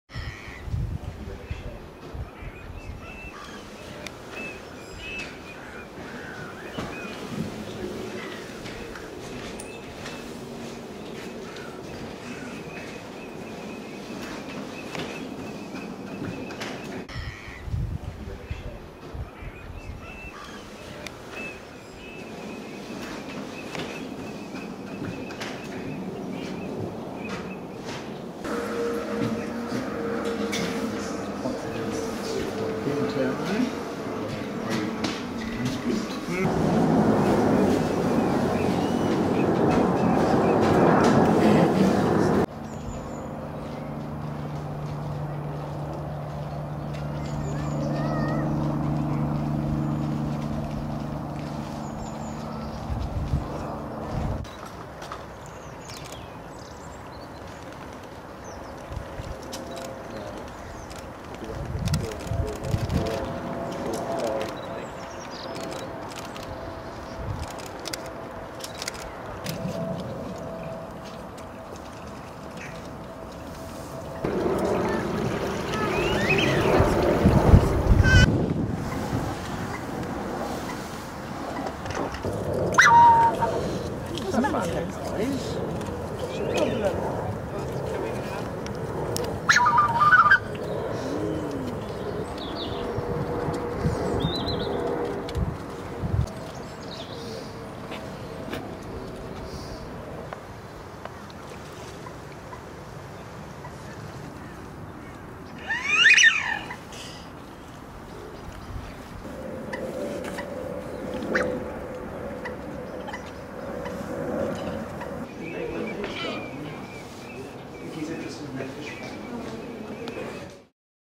Wildfowl & Wetlands Trust London
A wonderful day in London allows me to get some beautiful shots of the landscape. We see Shovelers, Pintails, Black Swans, Black-neck Swans, Green-Winged Teals, Puna Teal, Chiloe Widgeons